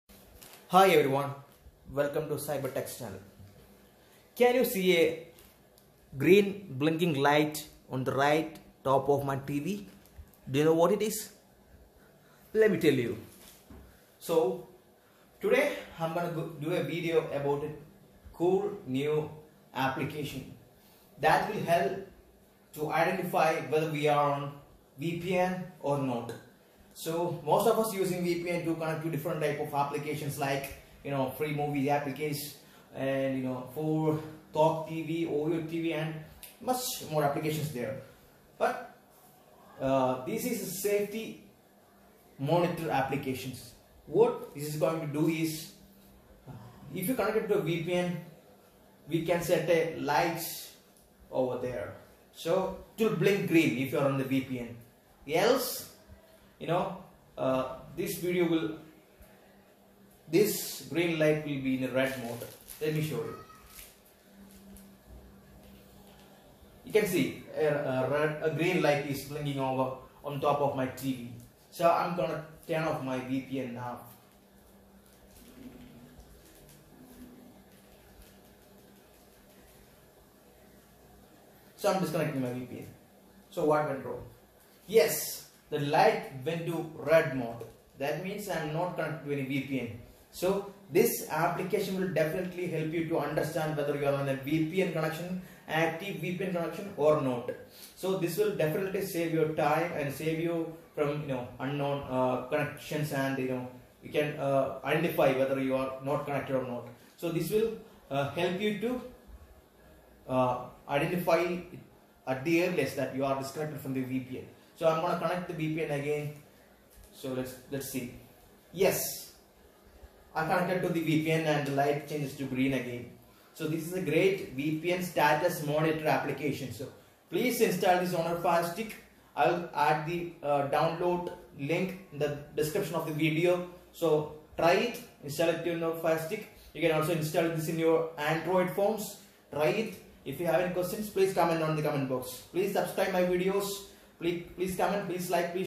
VPN SAFETY DOT - Are you sure you are connected to VPN ? 2020
Filelinked code: 81480504

VPNSafetyDot is an app for your FireTV or FireTV Stick which shows you if your VPN connection is active or not. It renders a visual indicator (dot) at the upper right corner of your screen. It doesn’t matter if you’re watching Netflix or doing something else in another app. The dot blinks green if your VPN connection is active. If your VPN connection is not active, the dot changes its color to red. This enables you to immediately see if your traffic is routed securely through your VPN provider or if you’re connected with your real ip address.

Does VPNSafetyDot include a VPN?
No! VPNSafetyDot is not a VPN Provider. VPNSafetyDot is only useful for you if you already have a VPN account from a provider like IPVanish VPN. VPNSafetyDot sits on top of your VPN connection and monitors it every few seconds and shows you the status on your screen. So there’s no need for you anymore to interrupt your streaming experience and switch to the VPN app to make sure your VPN is running. The only prerequisite is, that you already have a VPN that is installed on your FireTV or Stick.